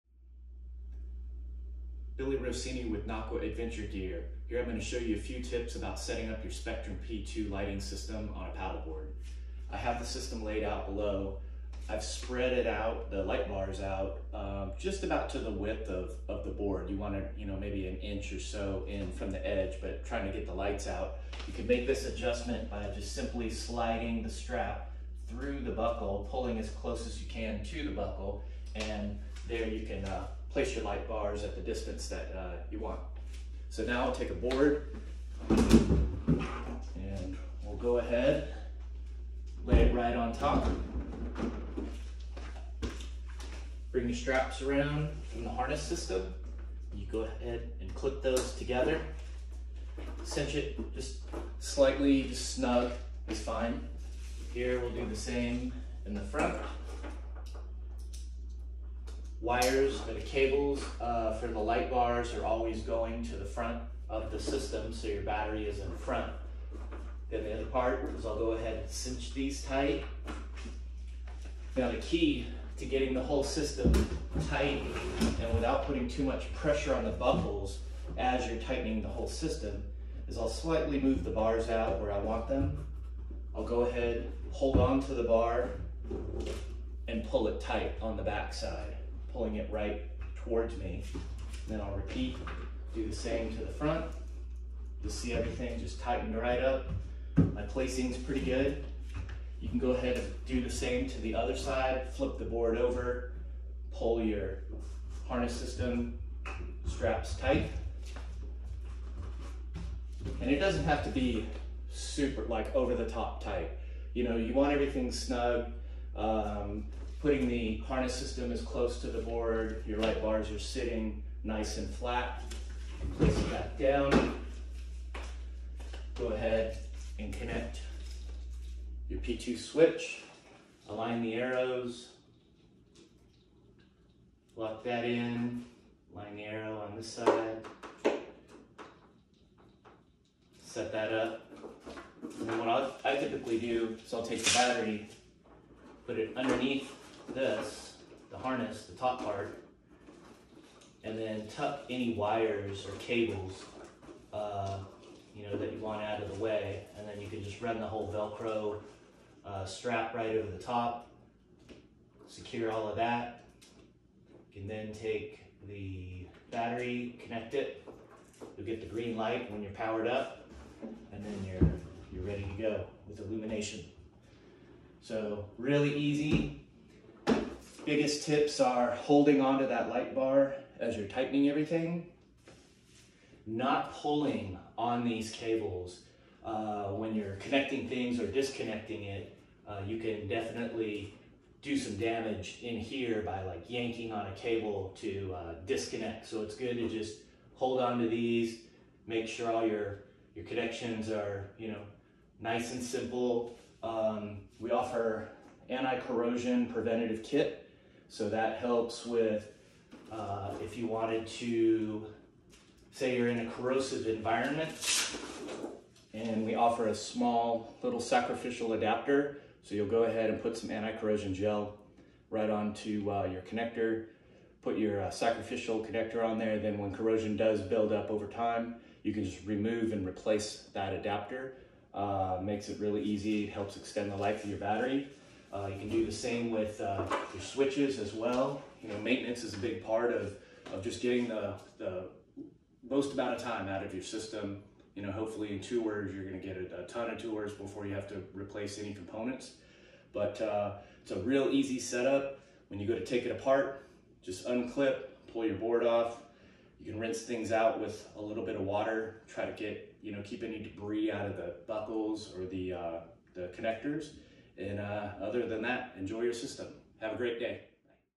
Nocqua Spectrum P2 Lighting set-up demonstration
Nocqua Adventure Gear's patented lighting system set-up demonstration video.